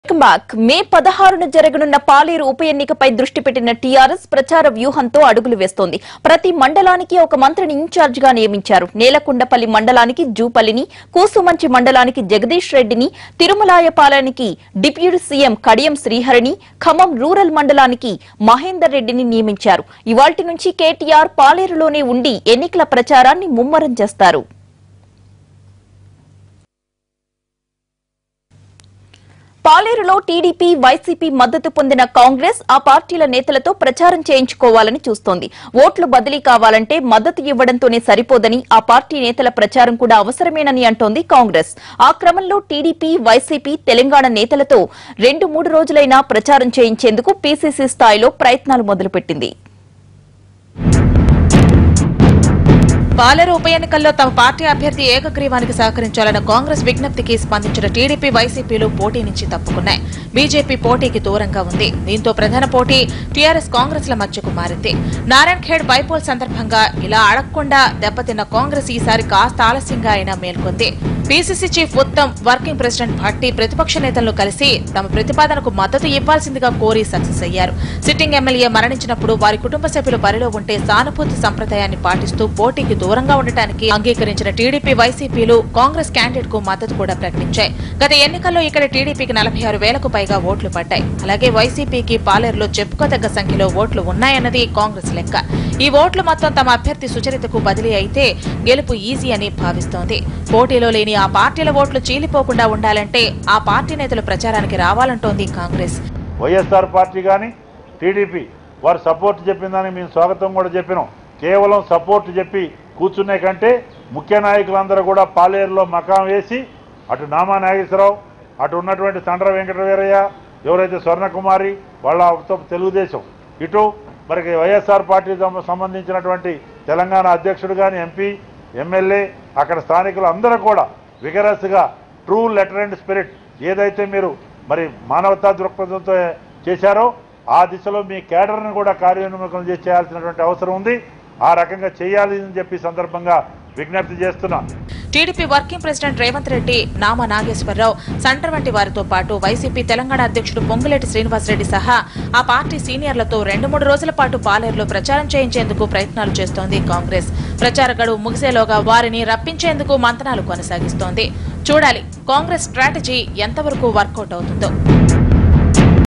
Congress To Get Help From TDP And YSRCP Parties In Paleru Election Campaigning | Latest Updates
ElectionsOnNTV Congress Party To Get Help From TDP And YSRCP Parties In Paleru Election Campaigning | Latest Updates